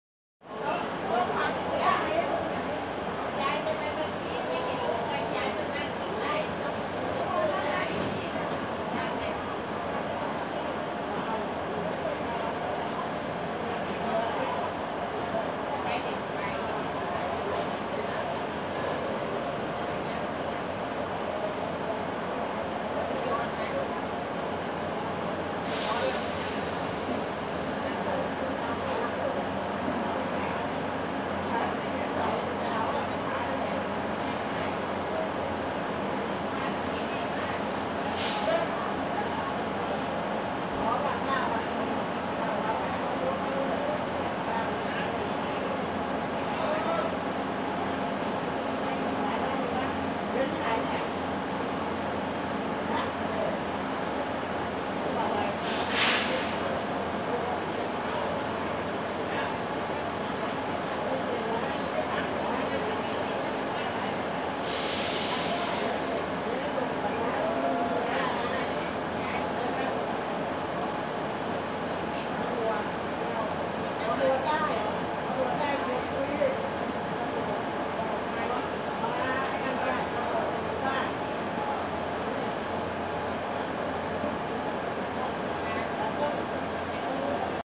كيفاش تيتغسل التران في التايلاند - This is how trains are washed in Thailand
كيفاش تيتغسل التران في التايلاند?
!This is how trains are washed in Thailand
Voici comment les trains sont lavés en Thaïlande!